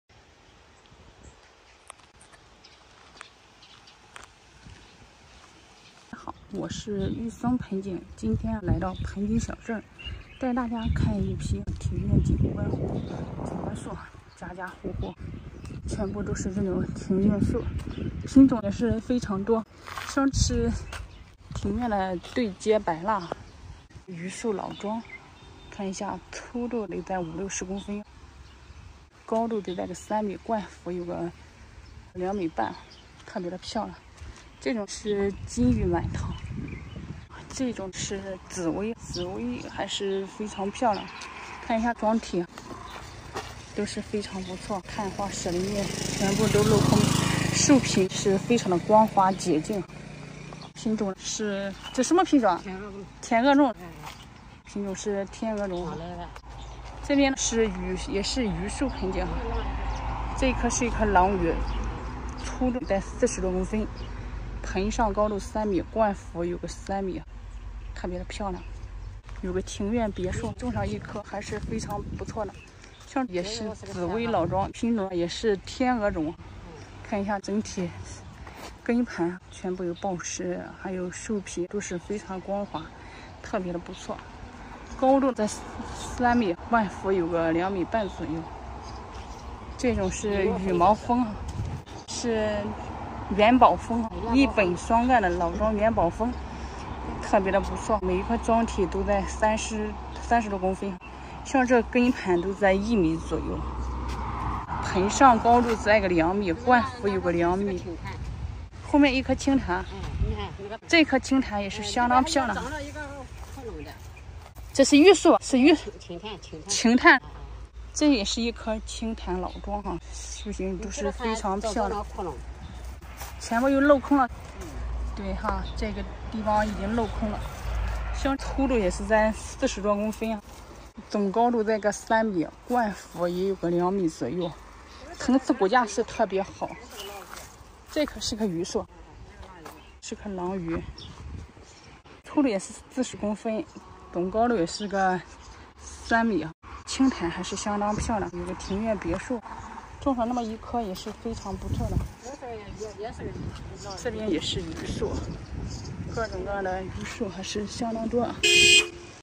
盆景小镇各种庭院景观品种多规格全生意难街上空无一人捡漏的时间
玉松盆景园艺
盆景是无言的诗立体的画，是生活的雕塑，交流与分享，喜欢盆景的老板们可以相互交流学习